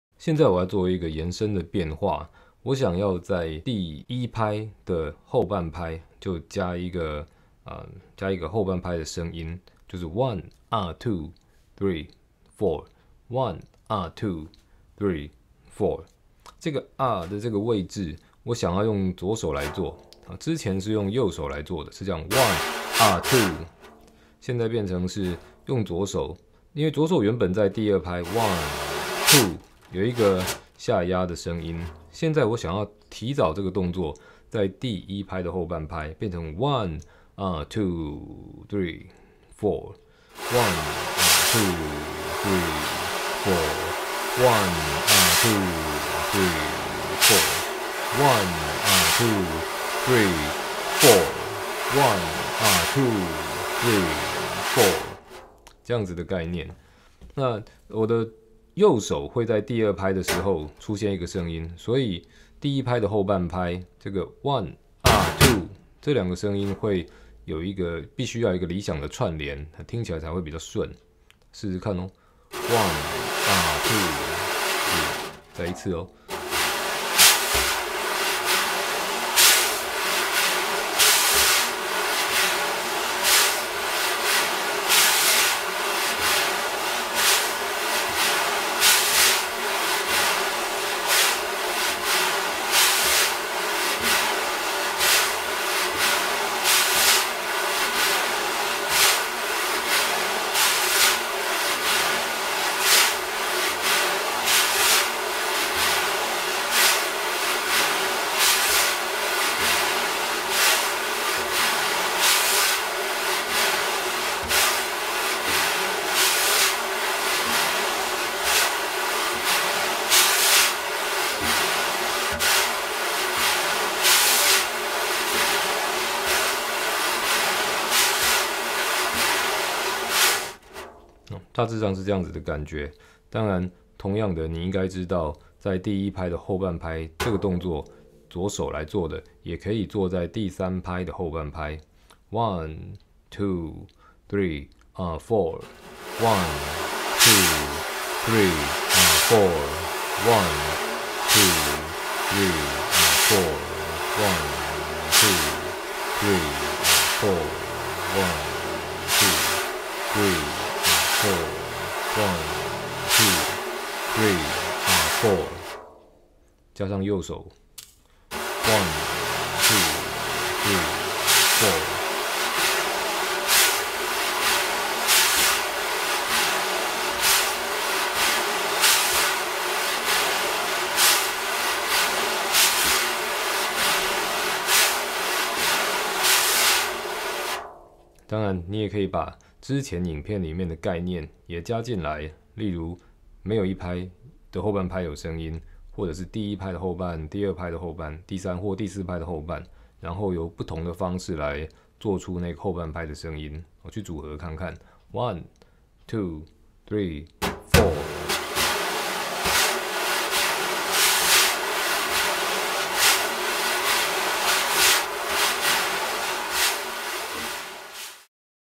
鼓刷教學[EP12]Swing Groove左手聲部1、3拍後半的時間點☆☆☆☆
影片中使用器材：
鼓皮Aquarian Modern Vintage Thin 14"
Peavey Radial Pro 1000 Snare